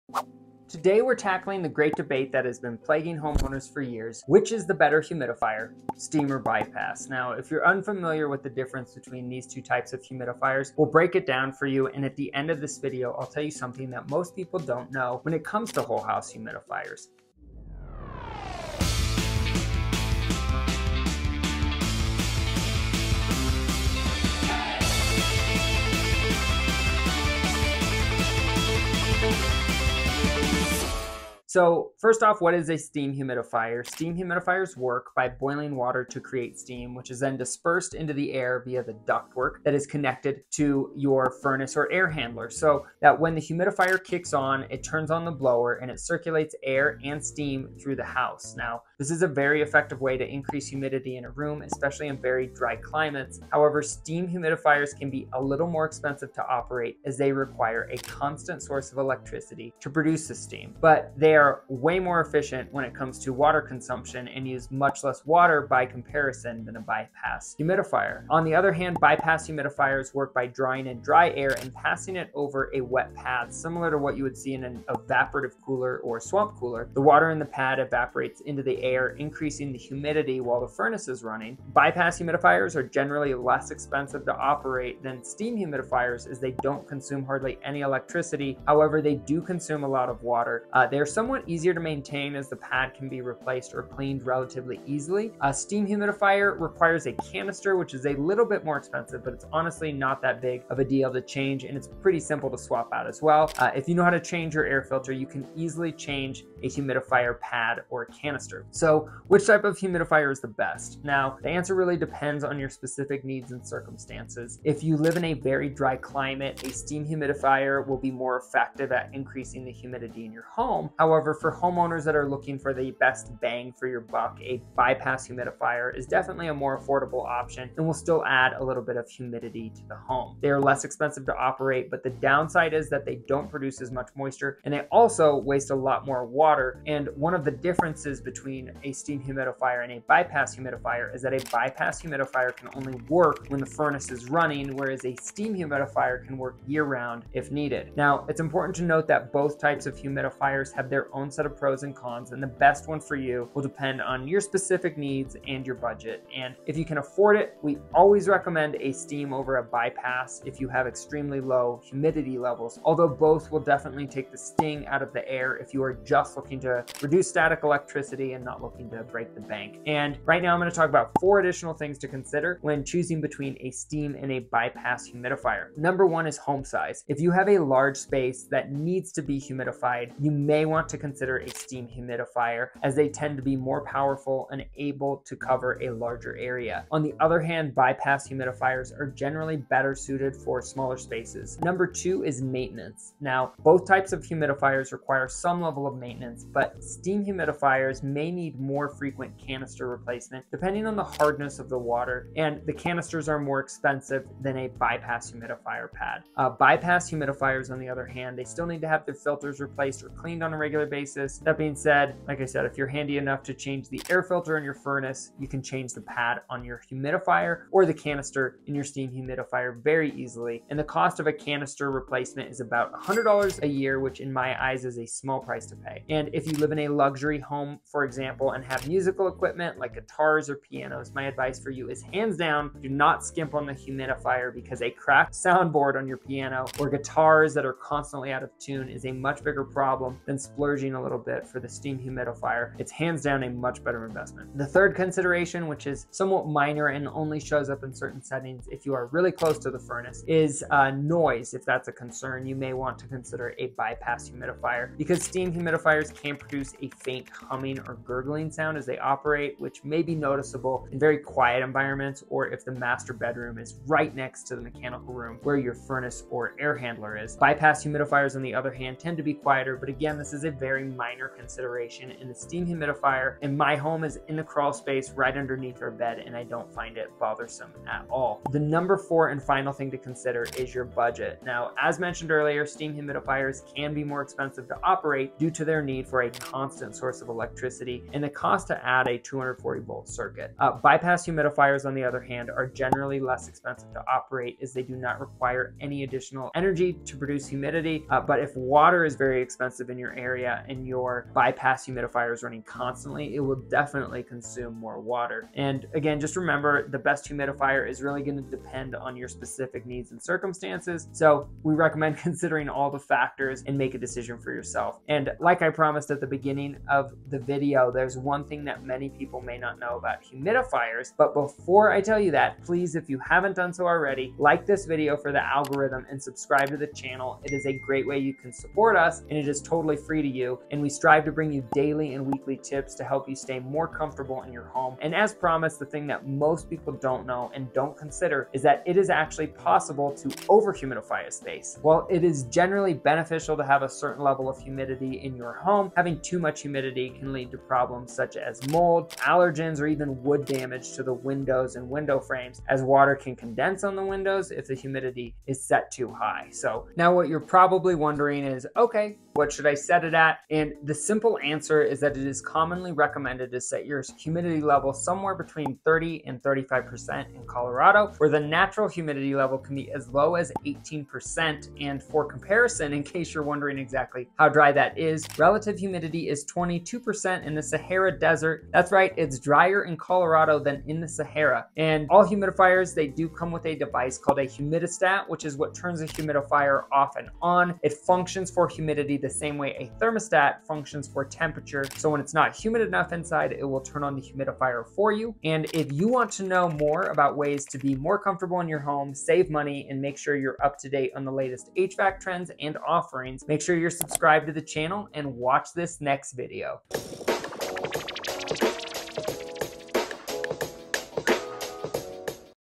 Humidifiers, steam or bypass? 4 things to consider BEFORE you buy… watch this FIRST 💯🔥
If you are interested in purchasing any of the humidifiers mentioned in this review we will link them here in the description if you are looking to self install any of these products. Don't forget to post a comment with any questions or requests :)

Link to our preferred Aprilaire 800 Steam Humidifier with Humidistat:

Link to our preferred Large Aprilaire 600 Bypass Humidifier kit (includes ductwork):

Link to our preferred Small Aprilaire 500M (Small) Bypass Humidifier kit: